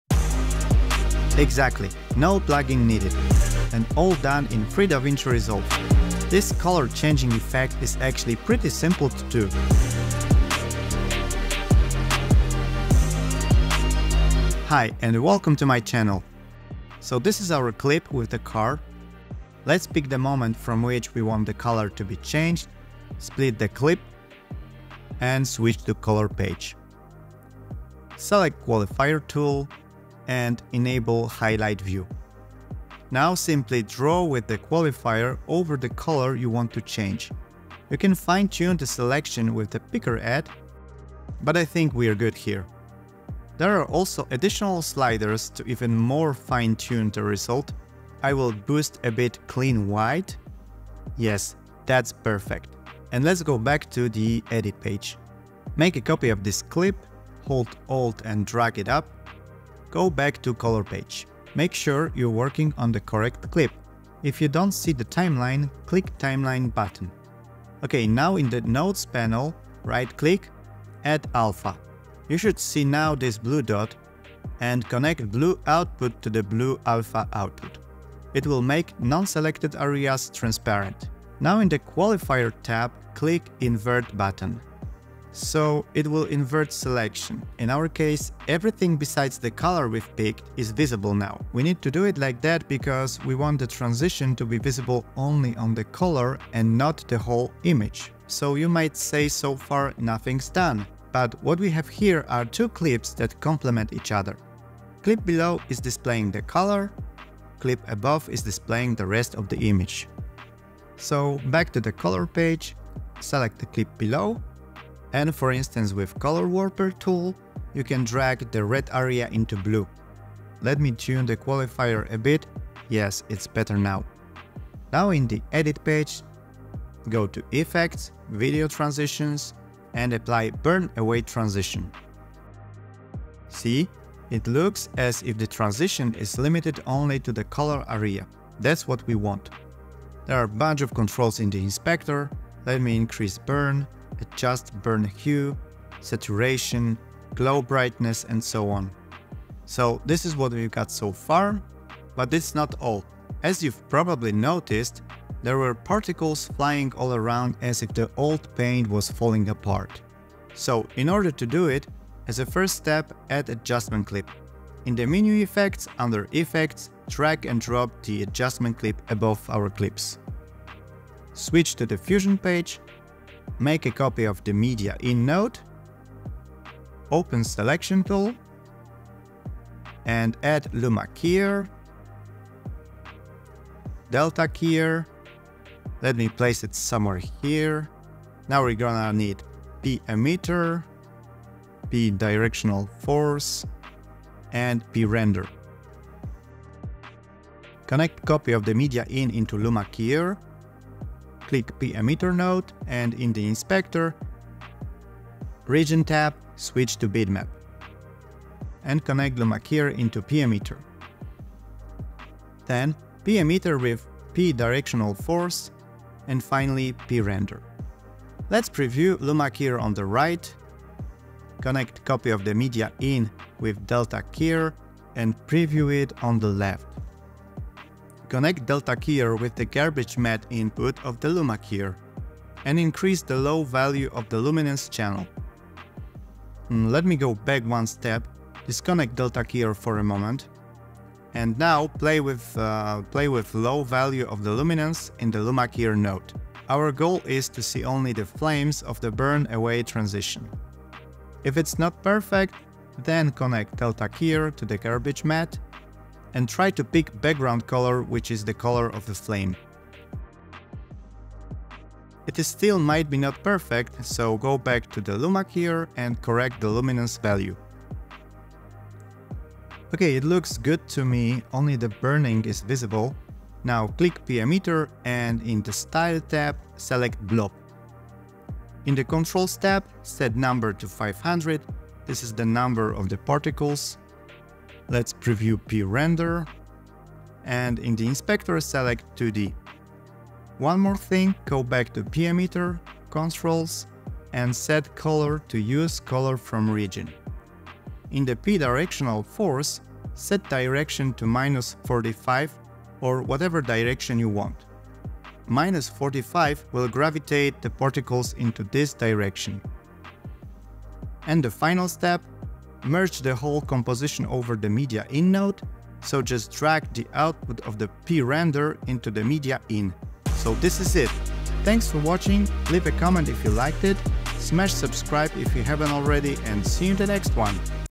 Spectacular Color Changing VFX in Free Davinci Resolve
In this video i show how to make a stunning color change effect in free davinci resolve.